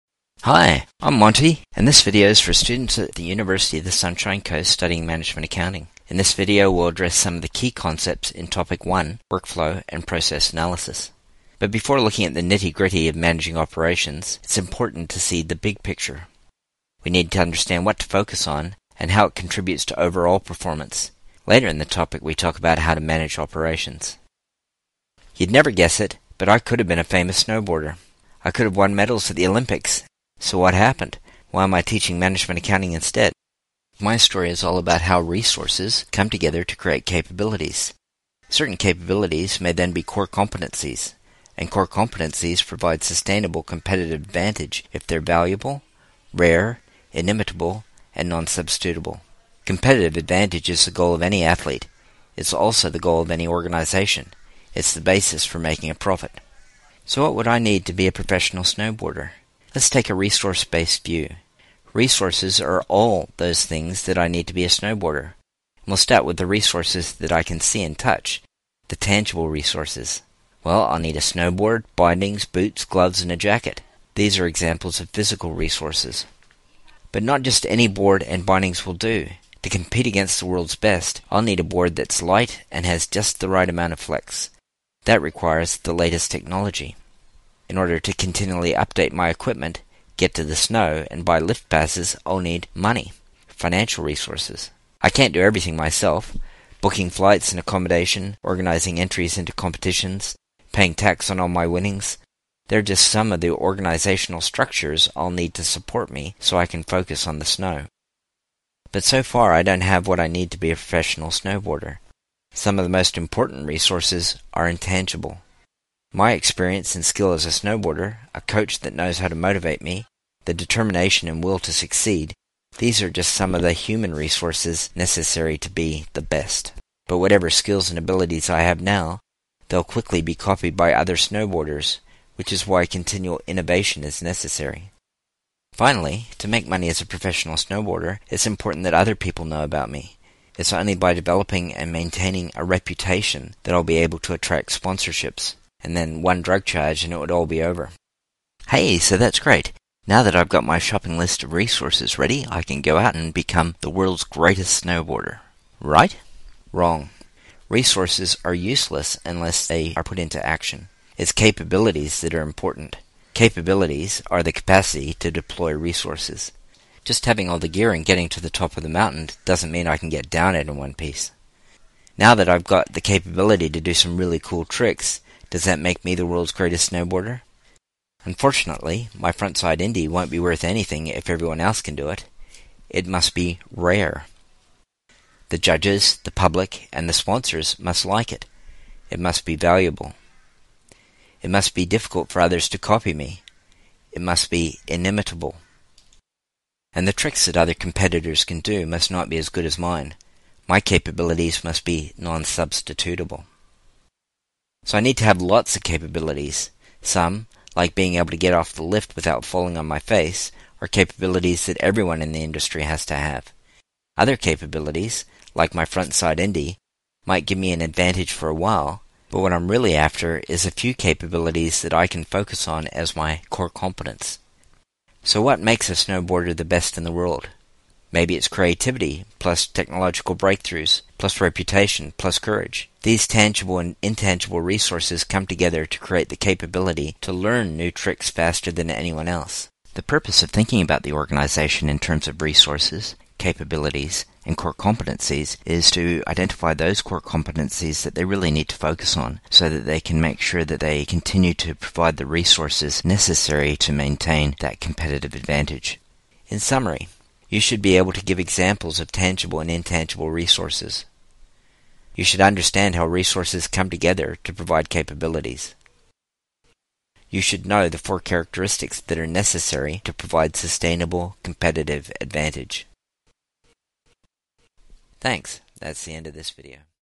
Core competencies
This video is for students at the University of the Sunshine Coast studying management accounting (ACC310 or ACC610). It is a brief overview of some of the issues that we deal with in the course.